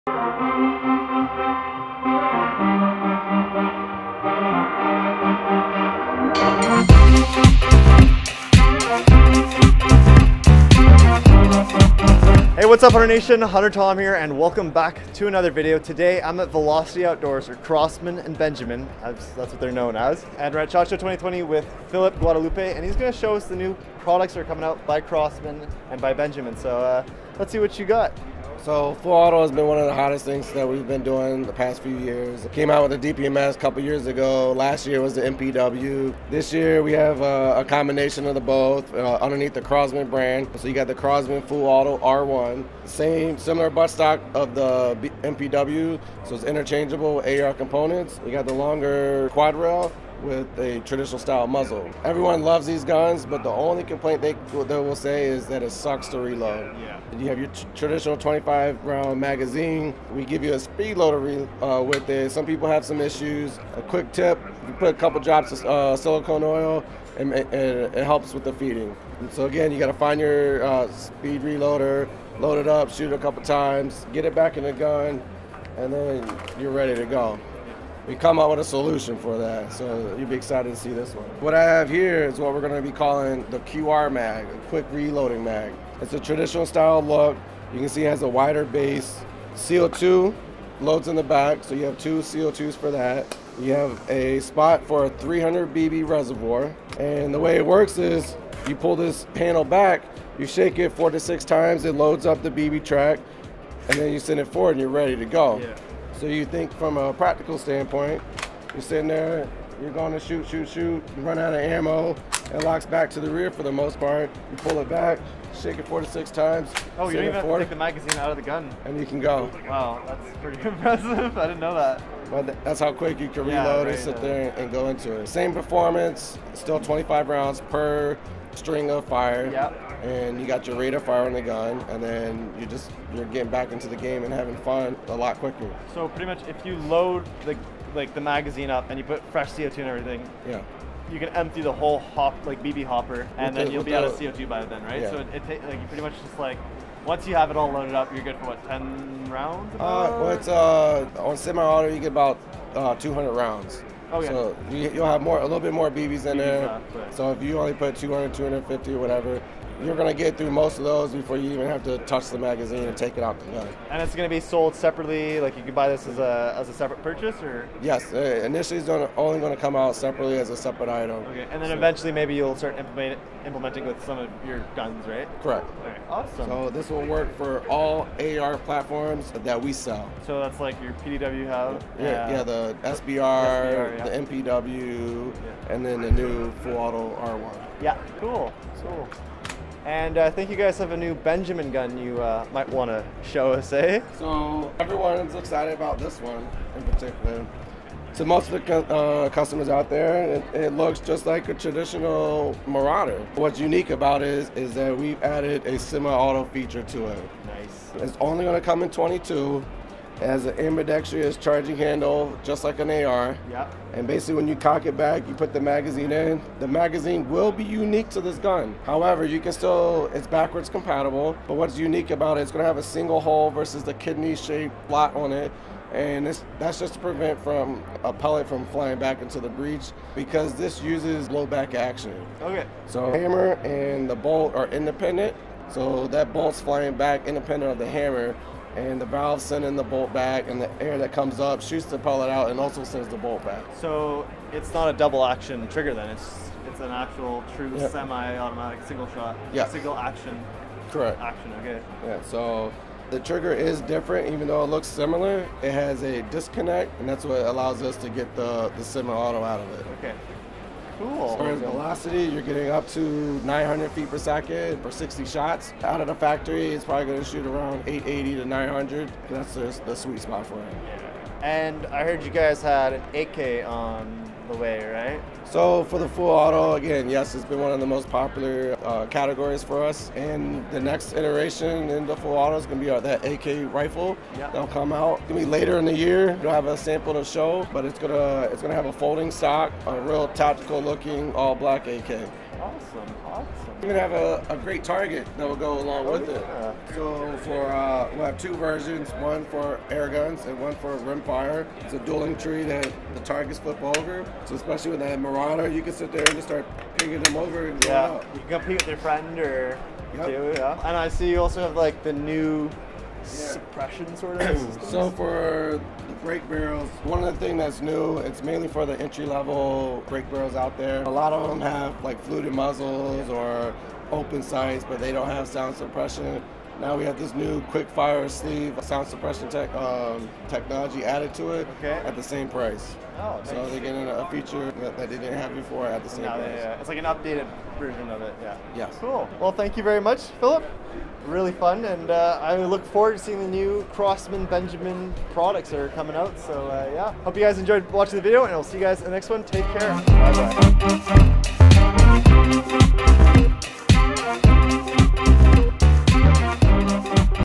NEW Crosman Airguns ShotShow 2020 | Semi-Auto Marauder PCP!!
Hey Hunter Nation! Today I'll be checking out what's new at the Crosman Airguns (Velocity Outdoors) booth at ShotShow 2020! Hope you enjoy!

@HunterTom x @CrosmanCorp